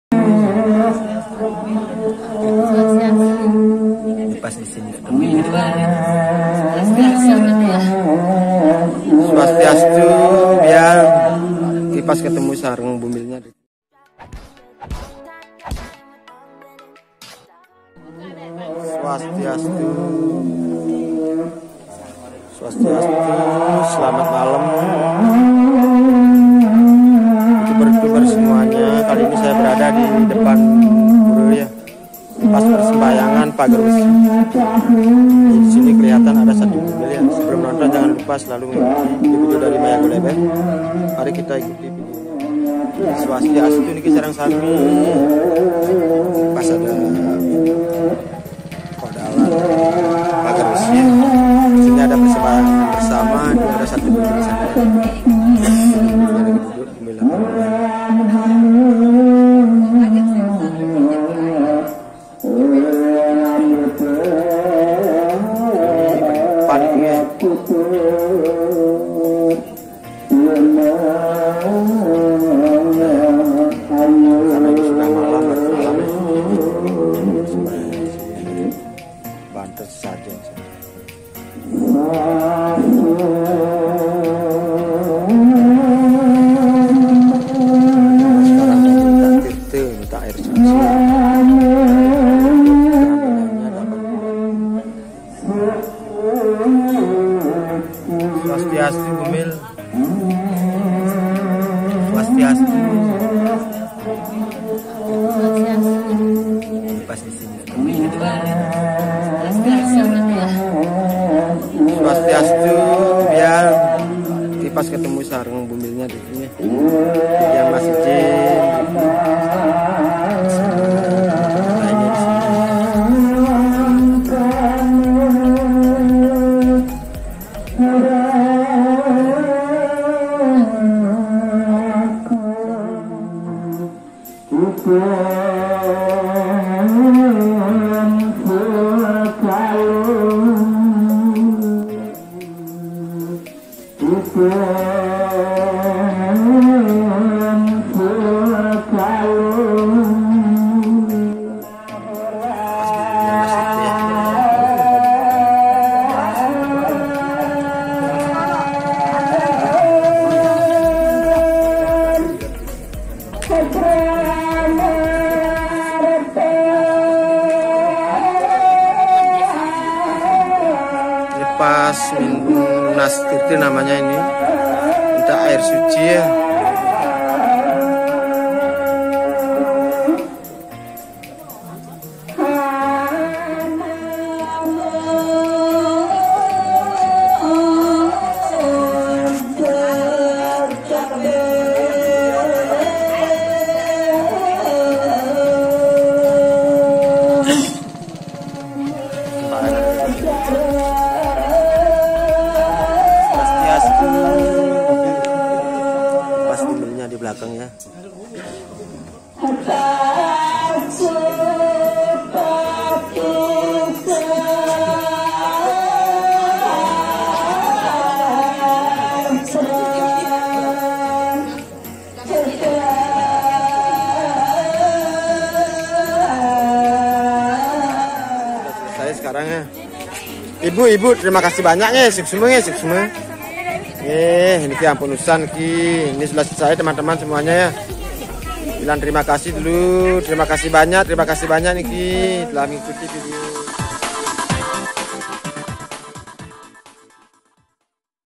BUMIL JUGA ADA//PERCIKAN AIR SUCI UNTUK BUMIL, DI HARI RAYA PAGERWESI@MAYAGOleber14
vidio berikut ini, bumil juga ada.//percikan AIR suci buat bumil, dihari raya pagerwesi. bumil juga sembahyang bersama. seperti vidio berikut ini@MAYAGOleber14